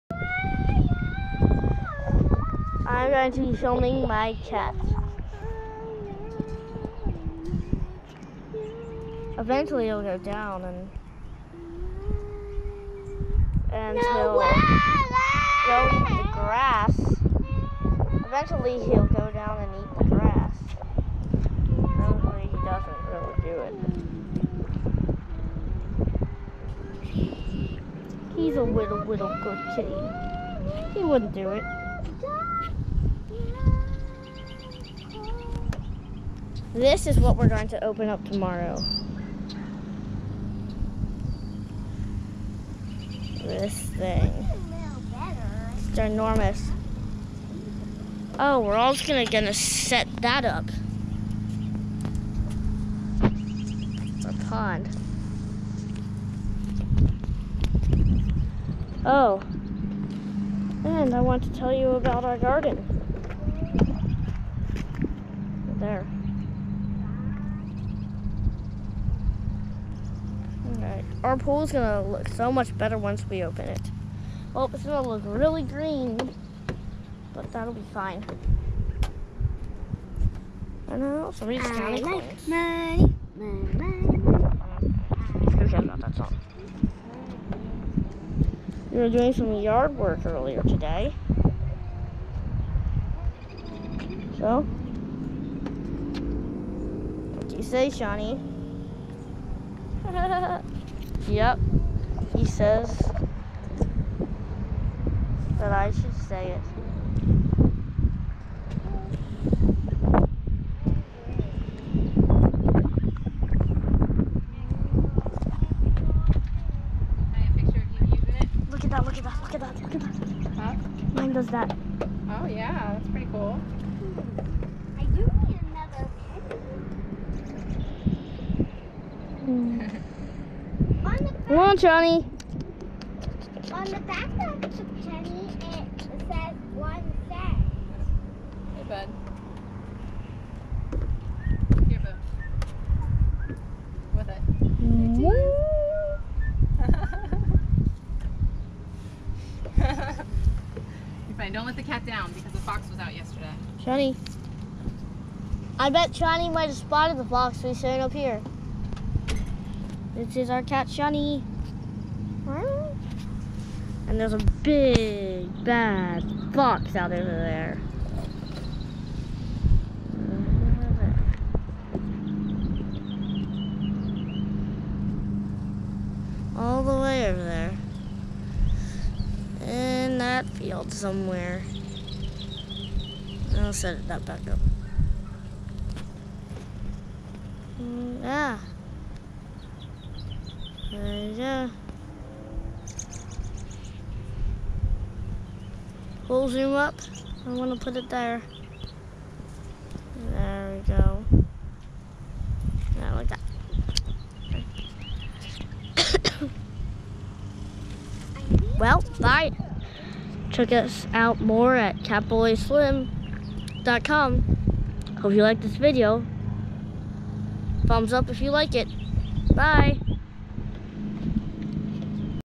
First vid with the new GoPro
A quick test of the new camera watching Sean John cat.